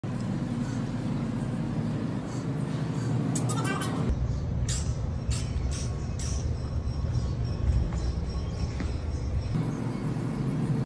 Cats Meat kha Rhi Hai In factory
Your Quries:-

Cats ko rozana meat dalty hai y hmari factory  m hi hoti hai